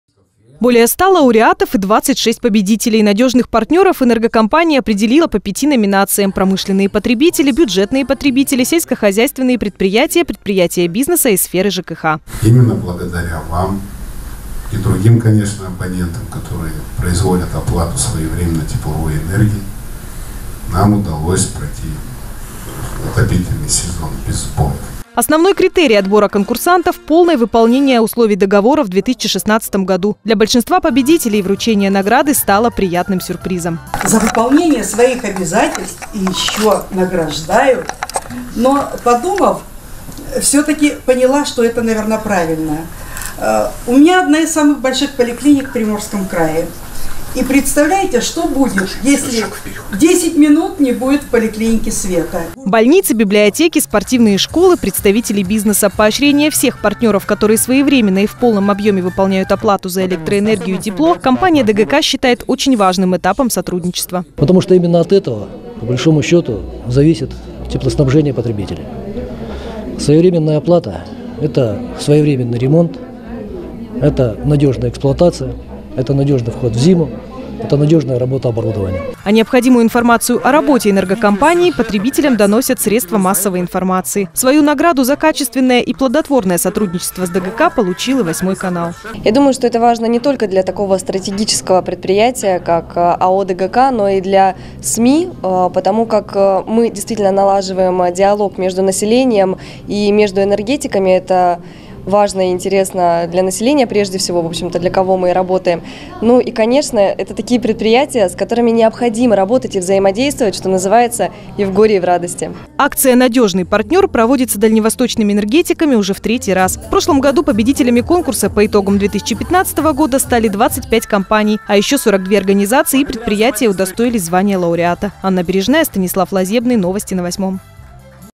Лучших потребителей тепловой энергии наградили во Владивостоке
«Дальневосточная генерирующая компания» наградила лучших потребителей тепловой энергии по итогам 2016 года. Рейтинг составили в рамках регионального этапа всероссийской акции «Надежный Партнёр». Впервые в ежегодном мероприятии участие приняли представители СМИ. Награду вручили и коллективу Восьмого канала.